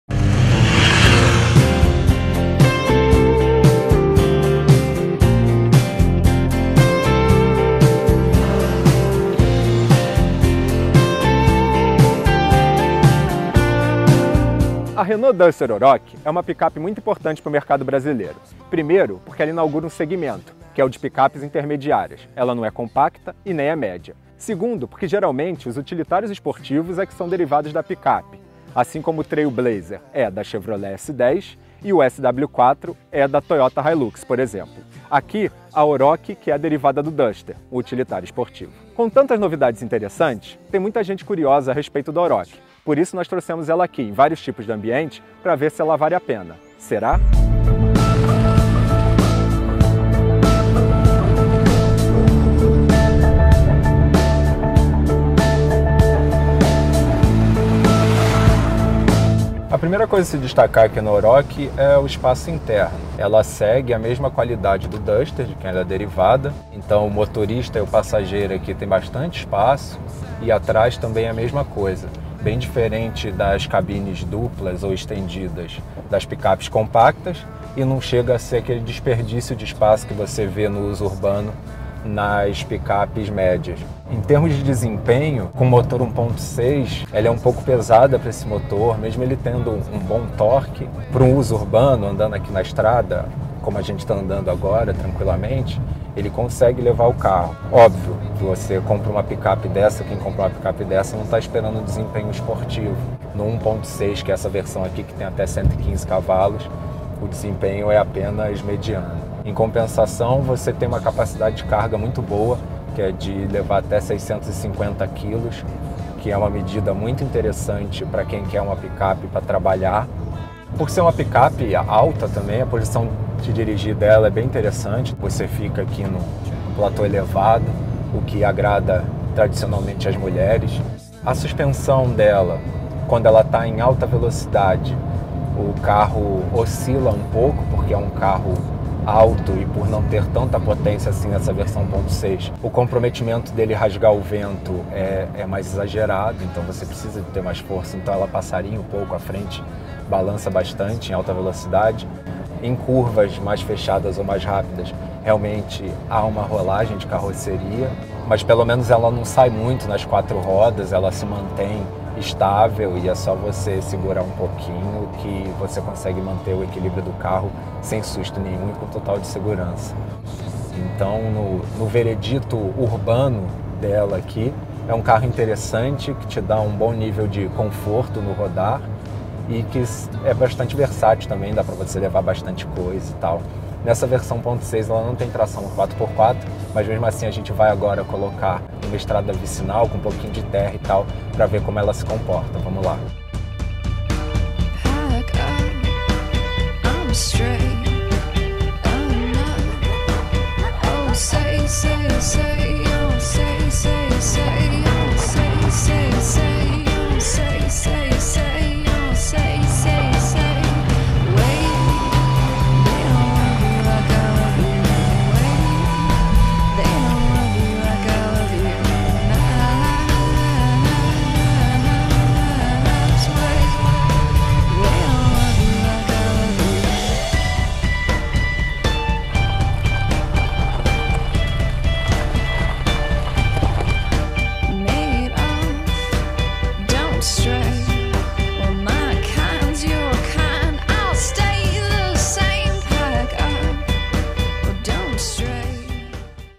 Aceleramos a Renault Duster Oroch
Colocamos o modelo à prova para ver se vale a pena investir em uma picape intermediária